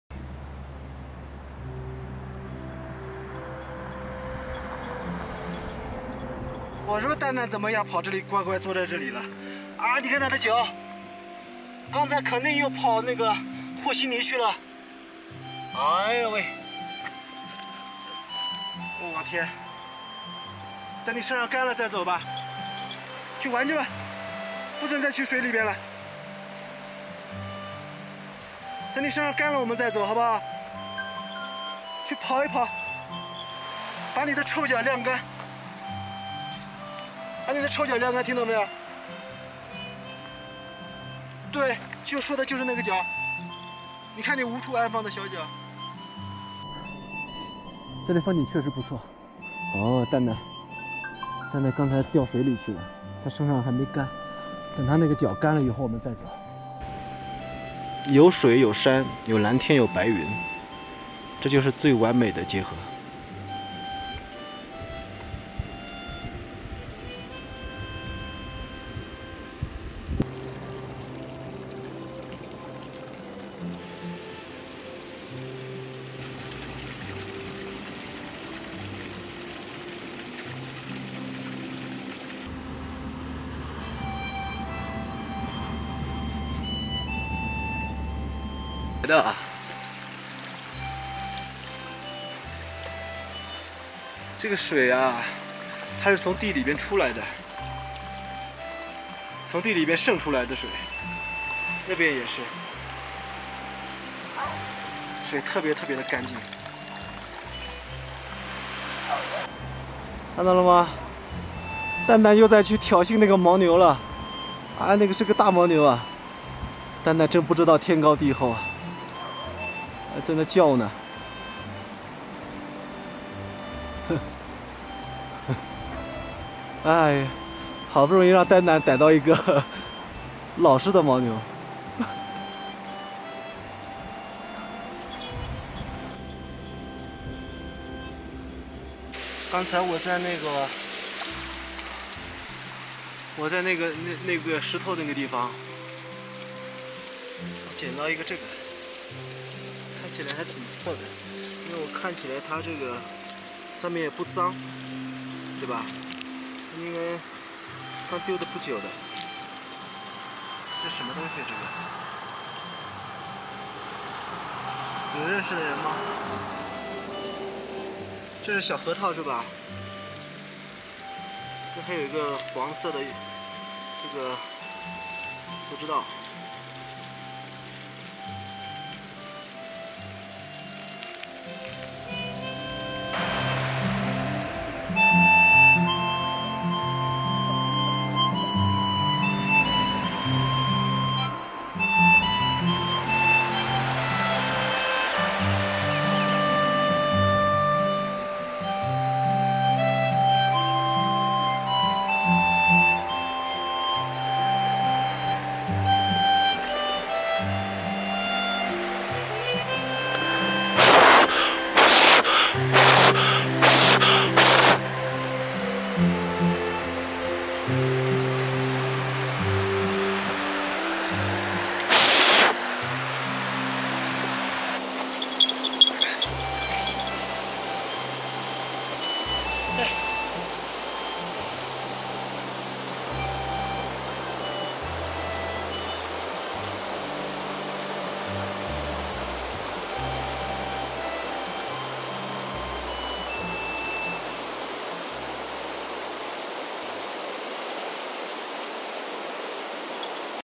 房车旅行中我在路边捡到一串小核桃，请问怎么吃？
今天在前往类乌齐的路上捡到一串小核桃，不知道怎么吃，直接砸开吗？